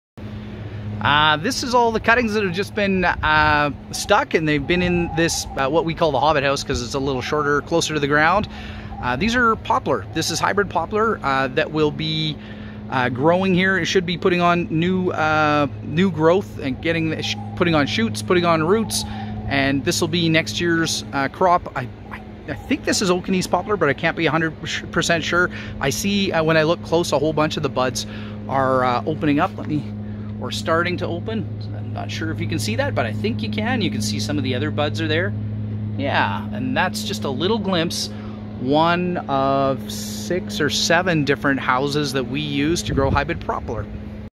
Hybrid Poplar (Okanese, Northwest, Brooks, Walker, Sundancer, Assiniboine) production at TreeTime.ca
As the largest propagator of Hybrid Poplar for shelterbelts and windbreaks in Canada, we've mastered the growing of this clonal crop from cuttings.

These are some of the fastest growing trees for the prairies (NE British Columbia, Alberta, Saskatchewan, Manitoba). If you want fast growth from your trees, think about planting hybrid poplar.